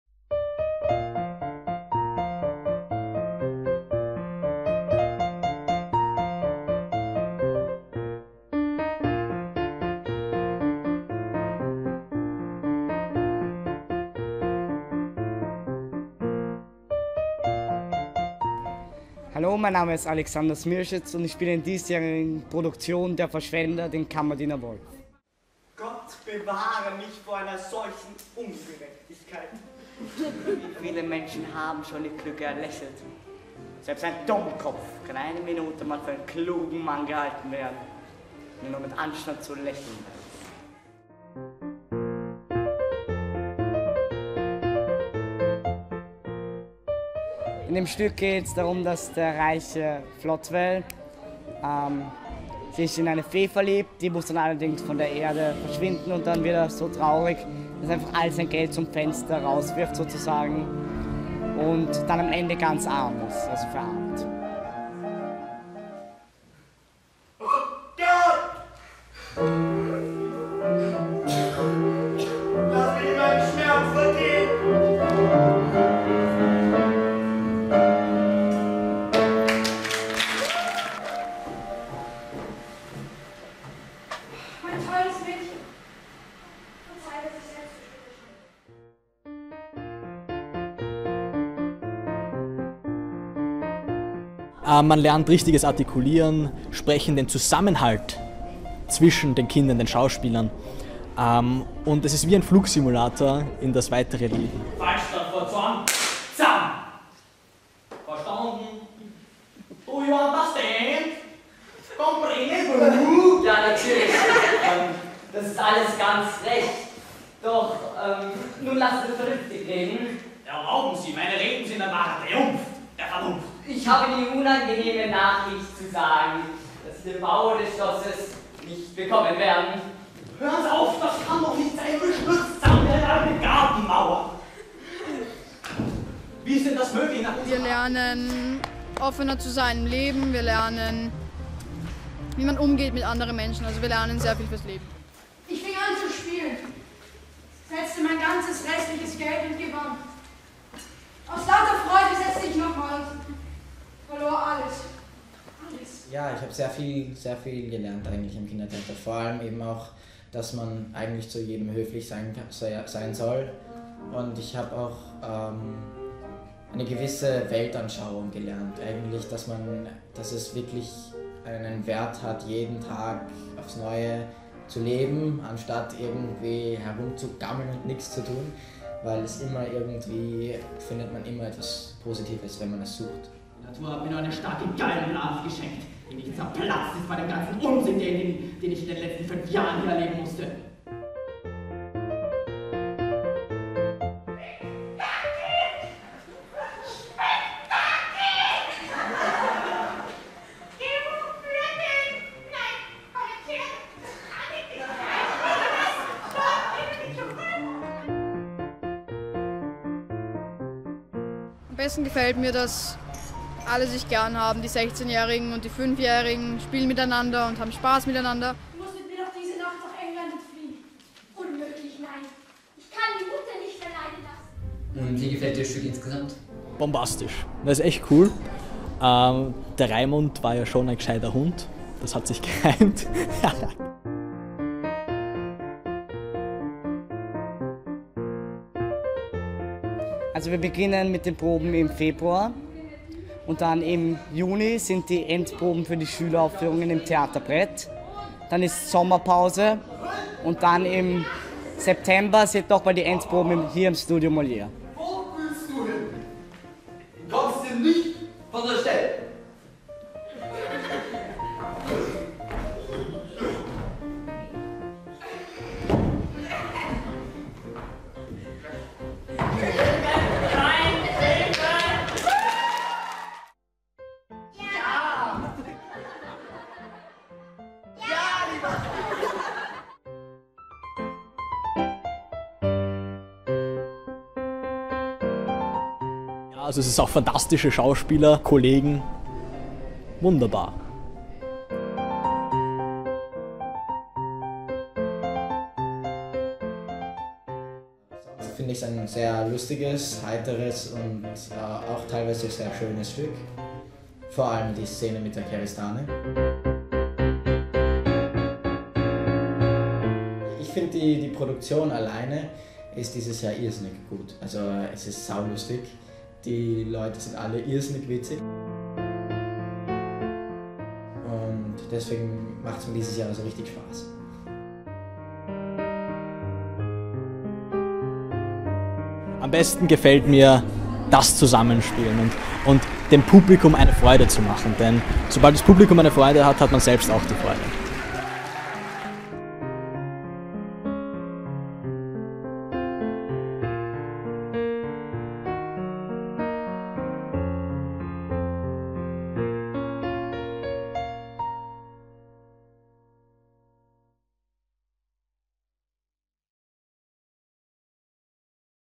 Trailer - Das Wiener Kindertheater 2012 - Der Verschwender
Das Wiener Kindertheater spielt "Der Verschwender" von Ferdinand Raimund.
Trailer